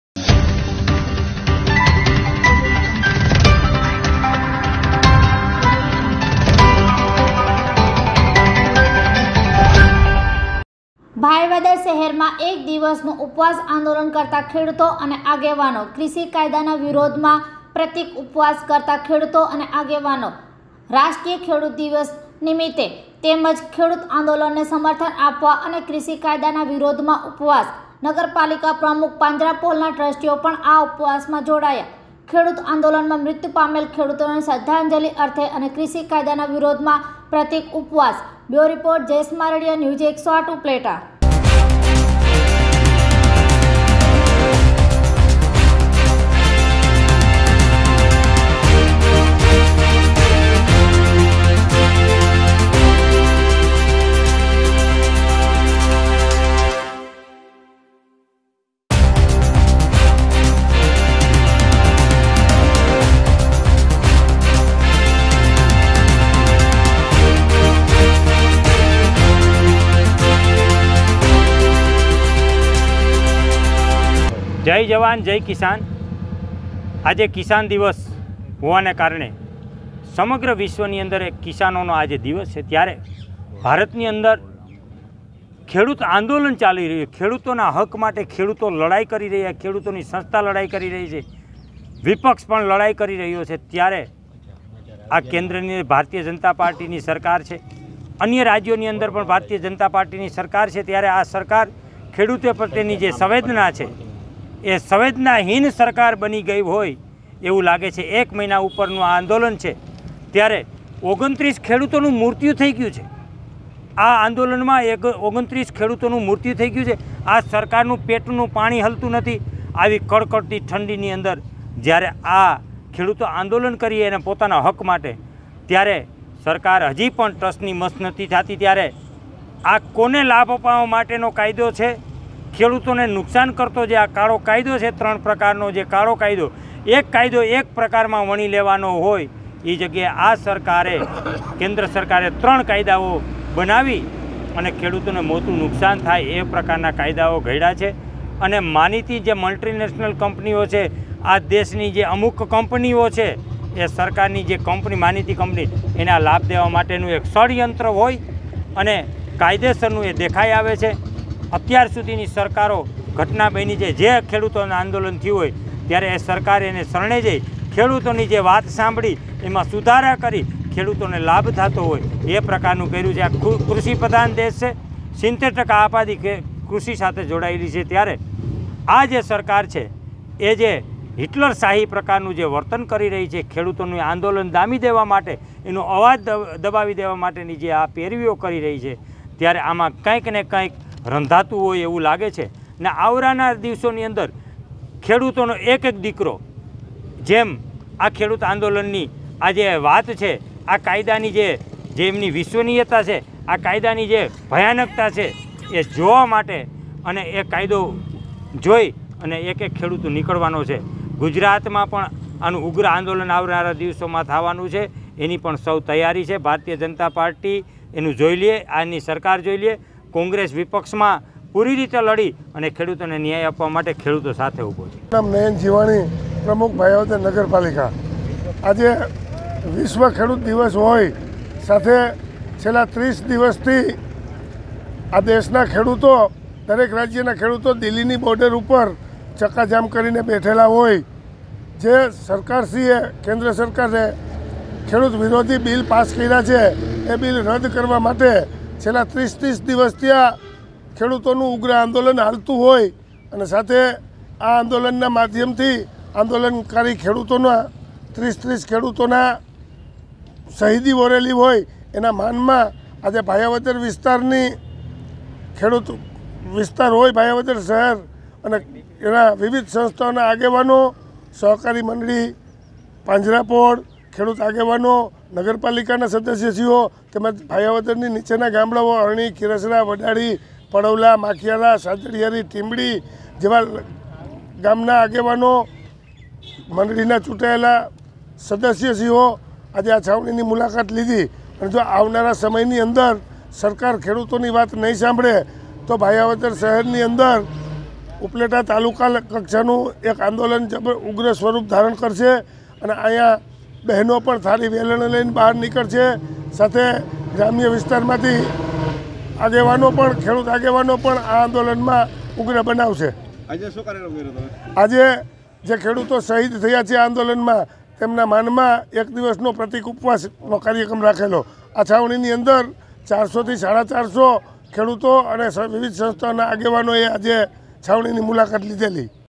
|ભાયાવદર શહેરમાં એક દિવસનું ઉપવાસ આંદોલન કરતા ખેડૂતો અને આગેવાનો NEWS108
ભાયાવદર શહેરમાં એક દિવસનું ઉપવાસ આંદોલન કરતા ખેડૂતો અને આગેવાનો